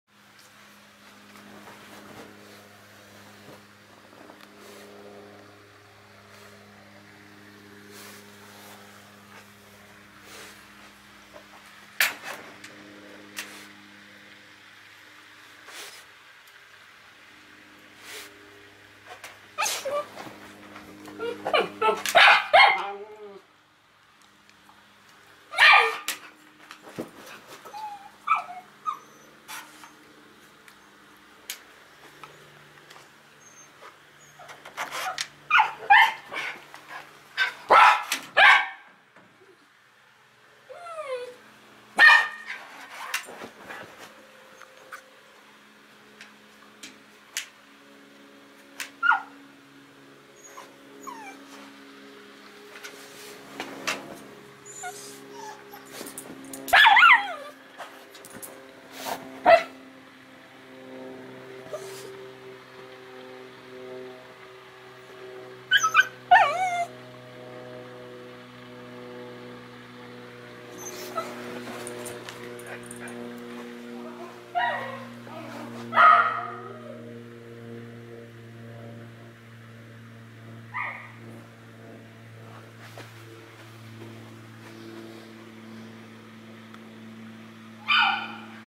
Basil and Ashton on the patio
Basil the fox terrier finds a bug and Ashton the Schnauzer get excited when she hears her mommy arrive home from work.